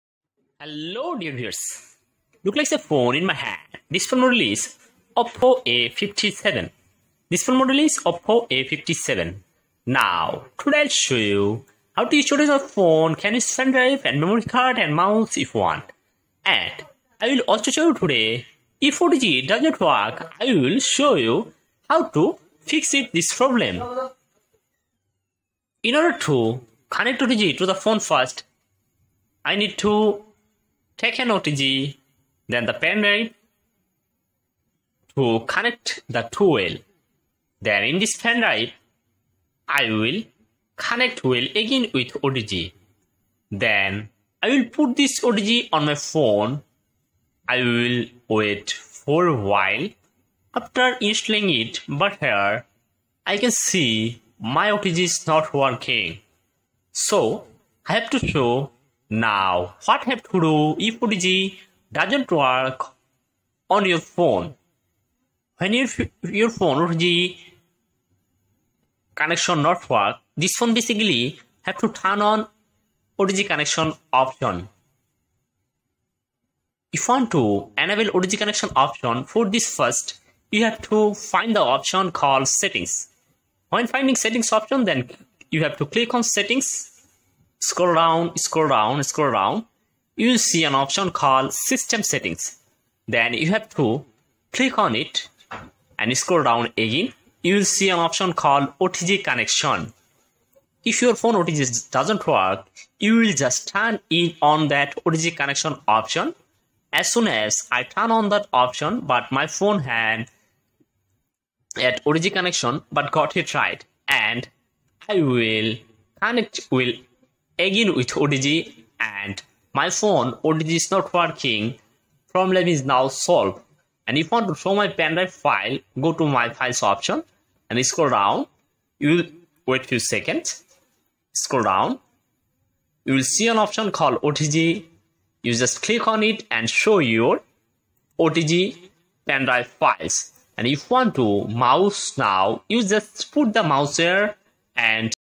Oppo A57 connect otg Use pendrive // Otg not working problem solve
Now today i will show you how to connect otg use pendrive in oppo a57 phone. and i will also show you today how to solve otg not working problem.